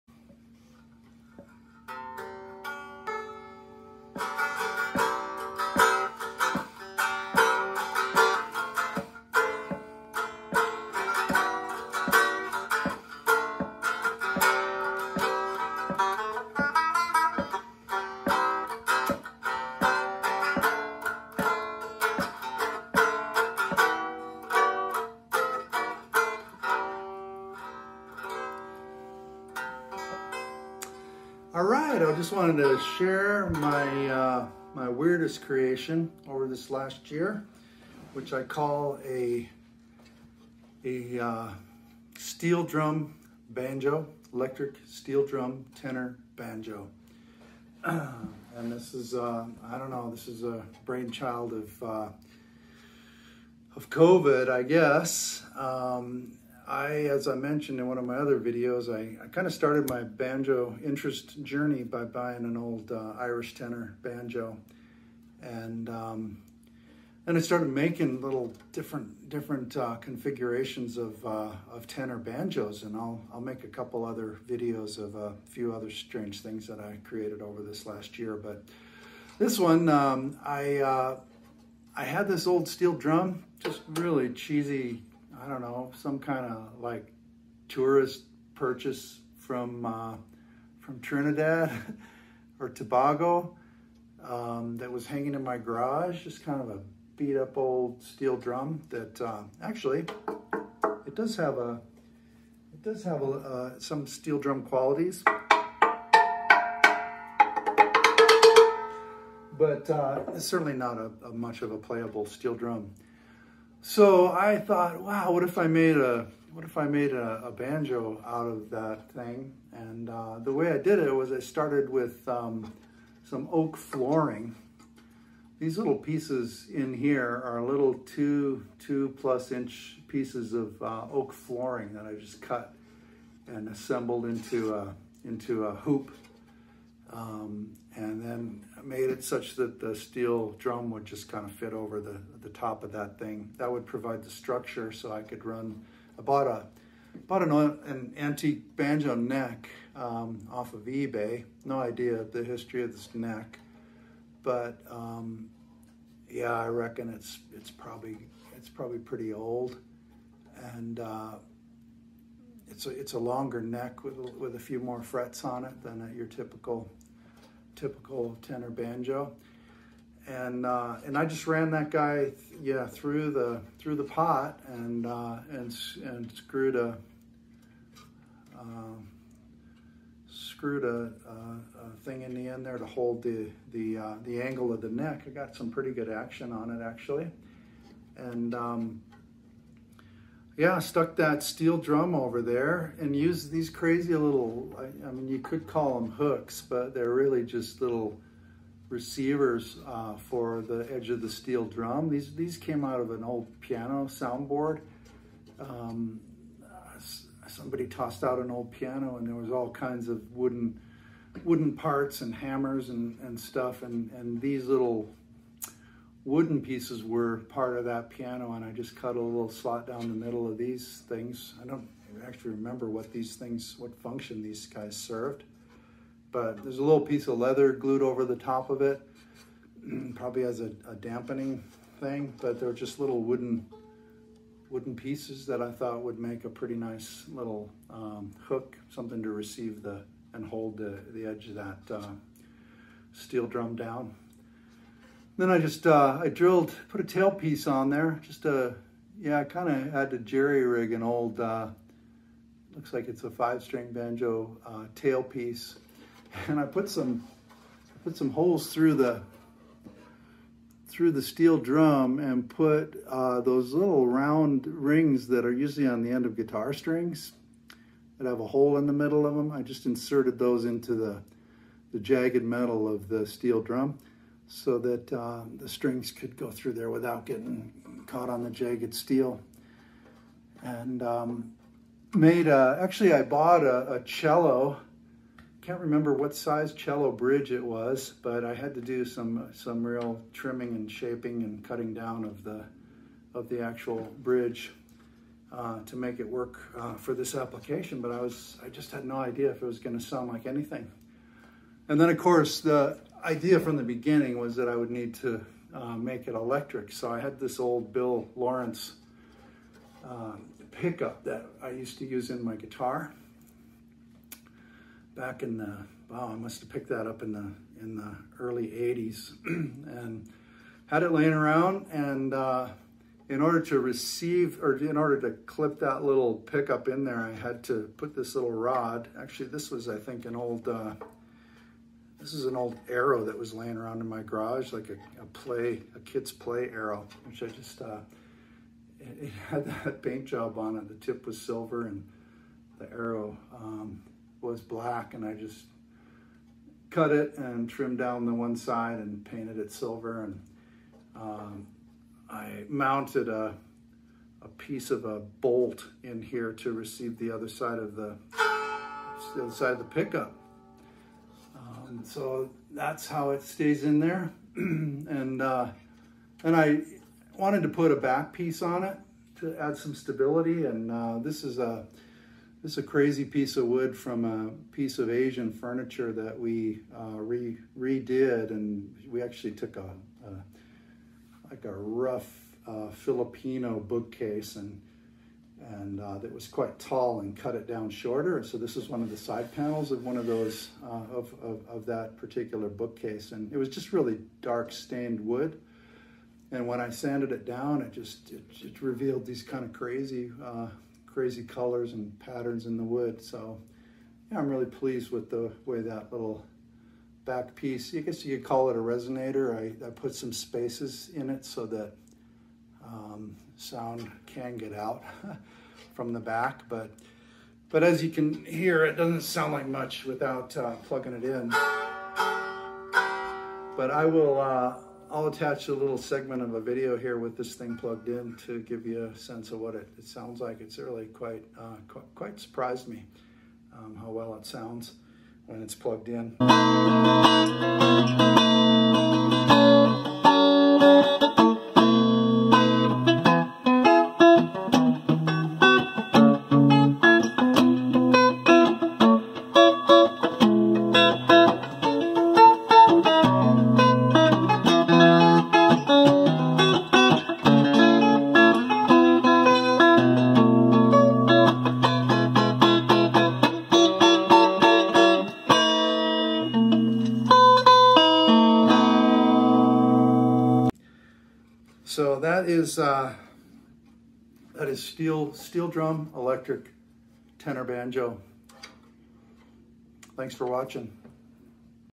Steel Drum Electric Tenor Banjo Build
How I built a tenor banjo from an old steel drum.